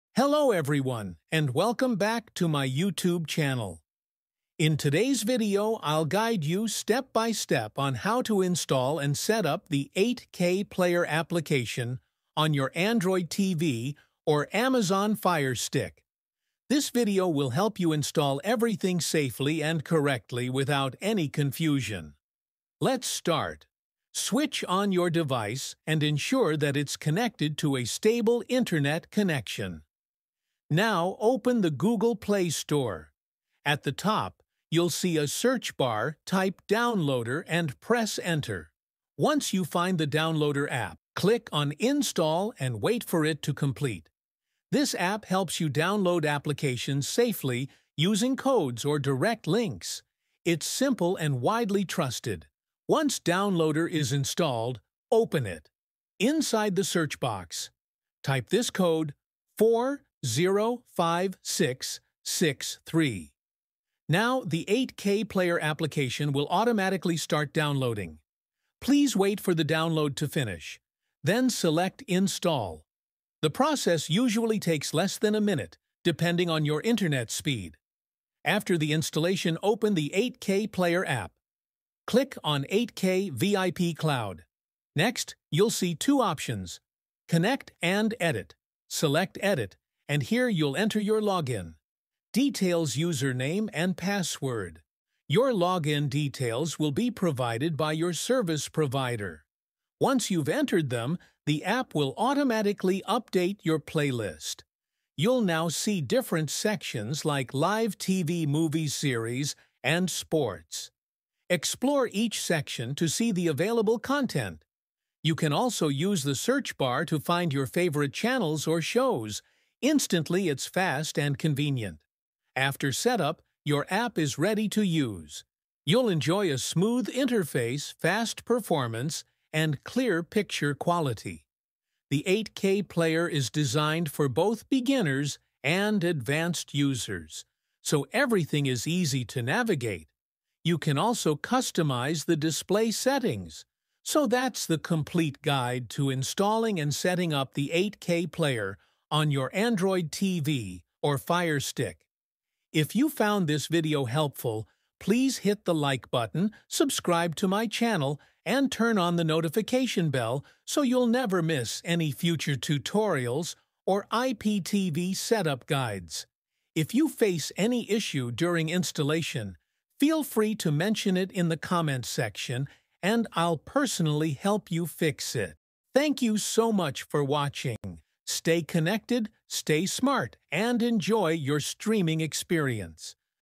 Download & Install 8K Player on Android TV & Fire Stick (UK | USA | NZ | Australia)
Welcome to Global IPTV Subscription Provider! 🇬🇧🇺🇸🇦🇺🇳🇿

In this video, I’ll show you how to easily download and install the 8K Player app on your Android TV or Fire Stick using the Downloader application.

📺 In this video, you’ll learn:
✅ How to install the Downloader app
✅ How to use the code 405663 to download 8K Player
✅ How to install and open the app
✅ How to watch Live TV, Movies, Series & Sports in high quality

💡 Note:
Your login details will be provided by your IPTV service provider.